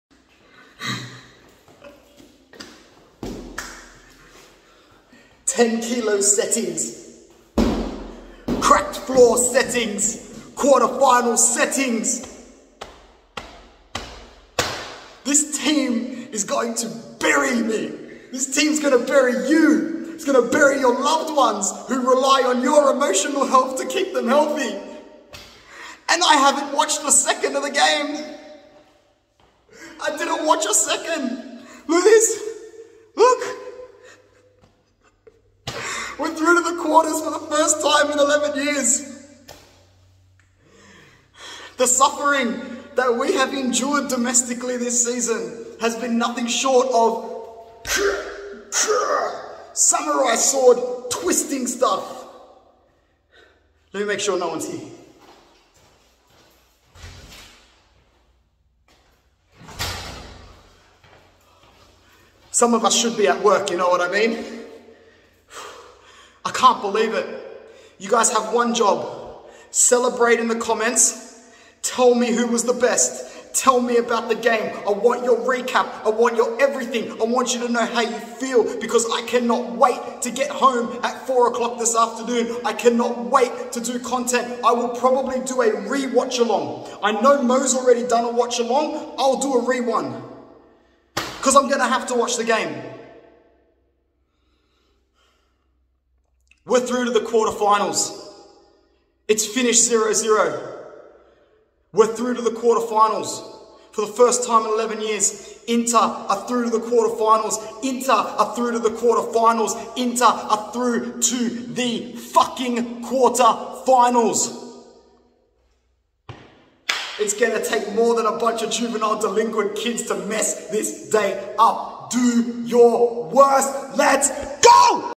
INTER ARE THROUGH TO THE QUARTER FINALS OF THE CHAMPIONS LEAGUE | EMOTIONAL ANTHONY
lost for words...